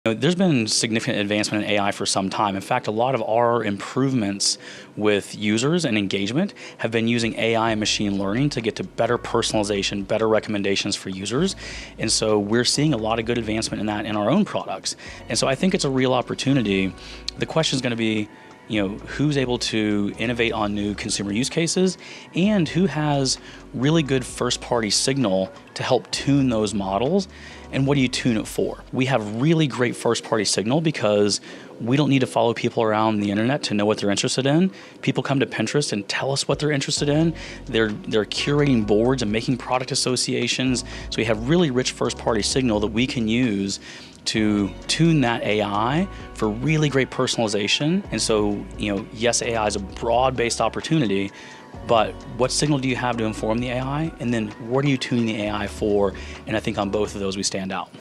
Pinterest CEO Bill Ready joins Yahoo Finance
Pinterest CEO Bill Ready joins Yahoo Finance to discuss  A.I. tech and how it’s boosting Pinterest’s platform with Yahoo Finance anchor and editor-at-large, Brian Sozzi, on February 7, 2023.